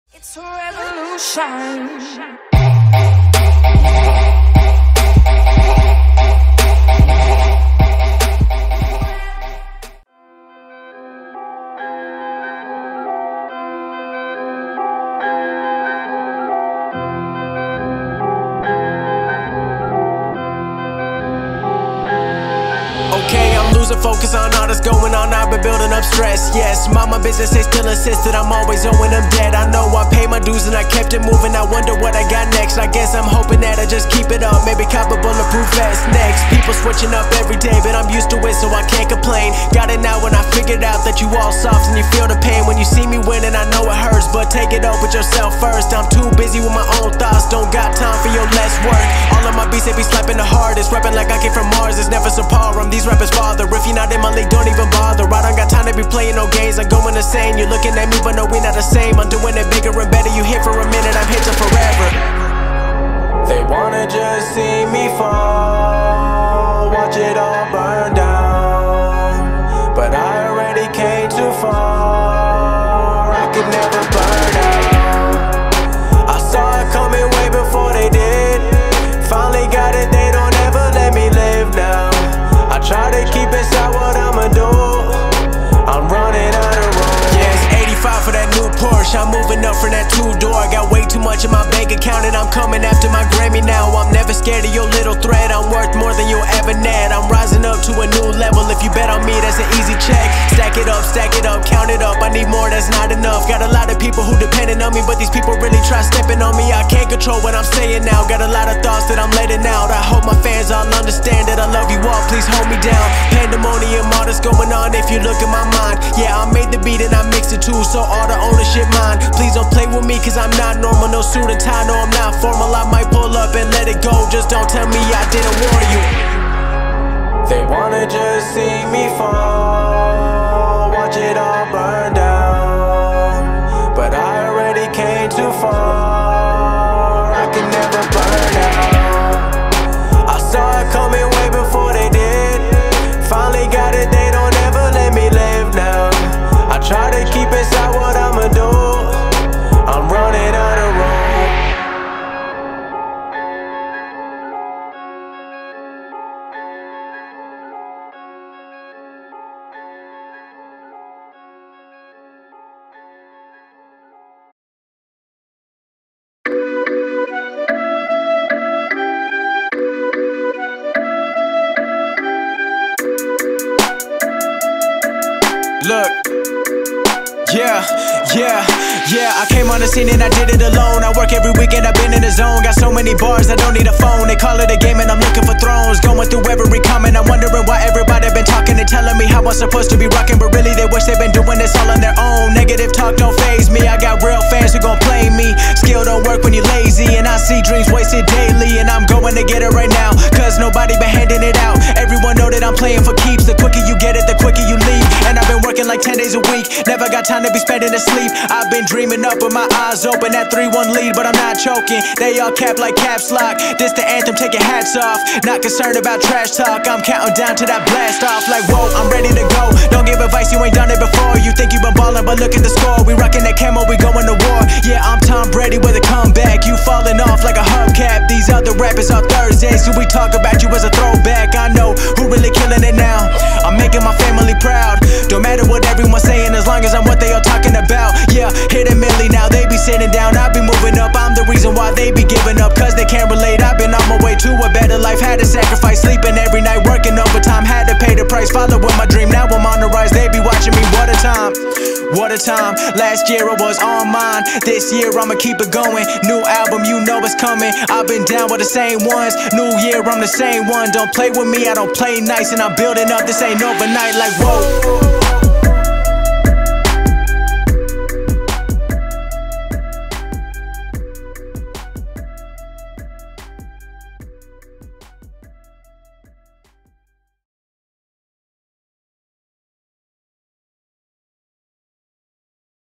MY REGIONAL QUALIFYING RUN!! - UNAA Area Qualifiers 2019
HI!! I'm back! I've been training for a while and getting back into it since October.  This was my first competition in 8 months and I qualified for UNAA regionals!  I'm super excited to be back and I'm glad you guys stuck around with me.  I really appreciate all of the support and let's see how I do this season.  I have more projects coming soon and this is a good way to end 2019.  :)